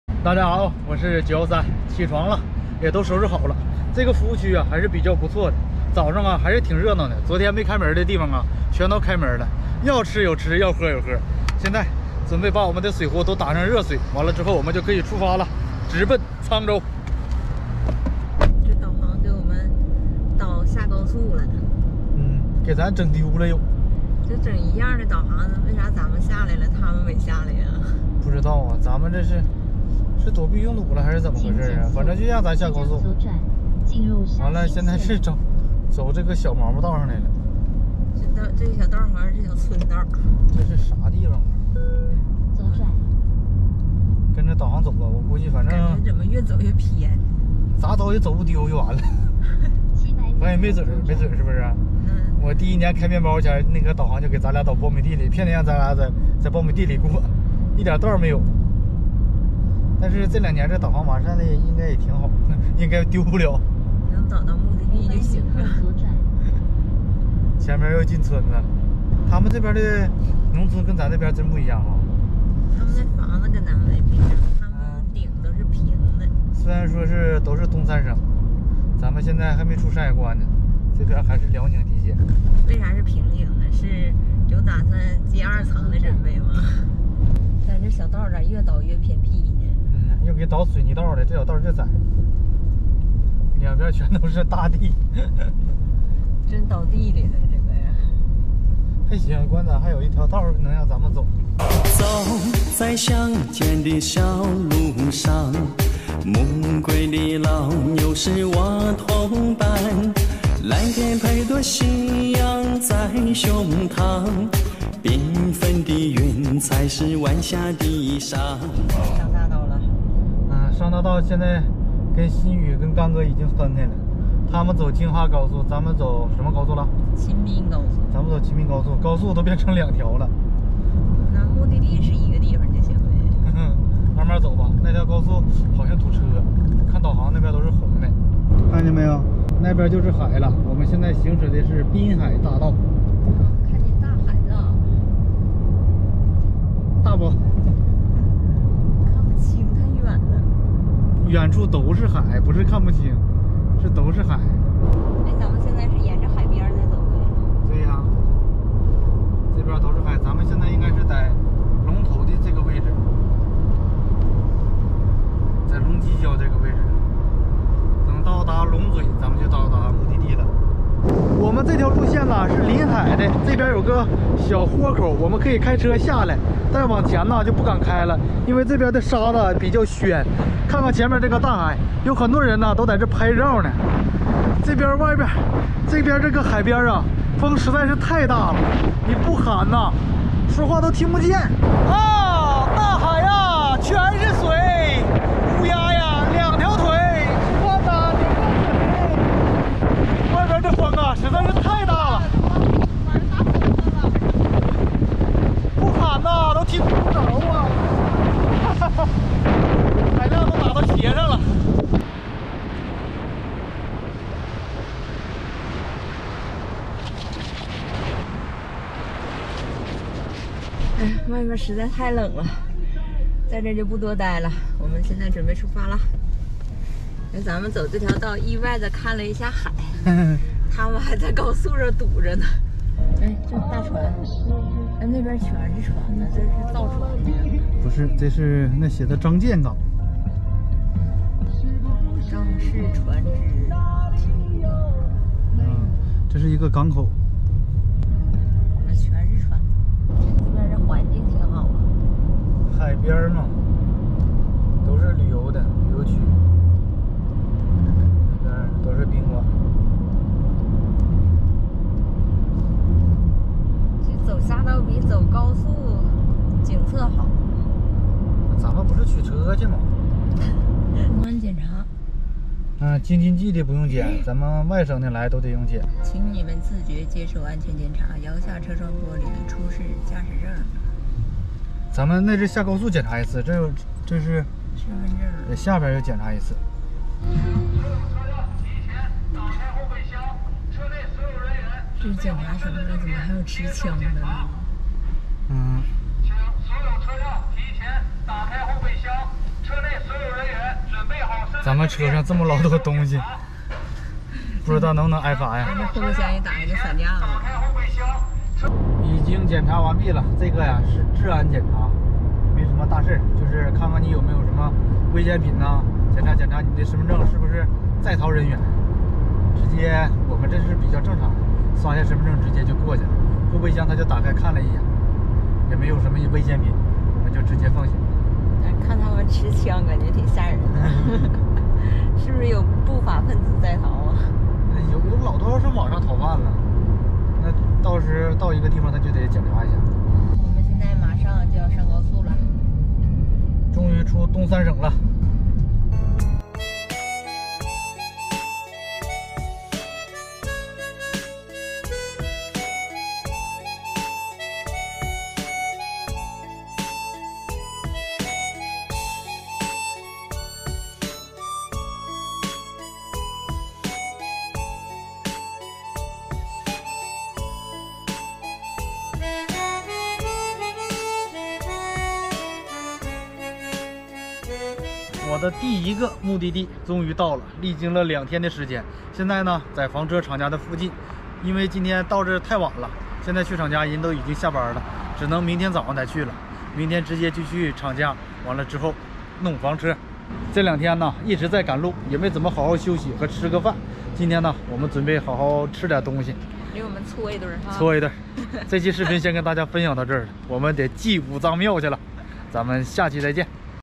被导航带到了乡间小道，上了大道又被“持枪劫道”这一路太精彩了【环华行913】
大家好，我是东北小哥913，
公司停业决定跟馨予学习自媒体，
我将在这里记录分享我的生活，
请大家多多支持我！！！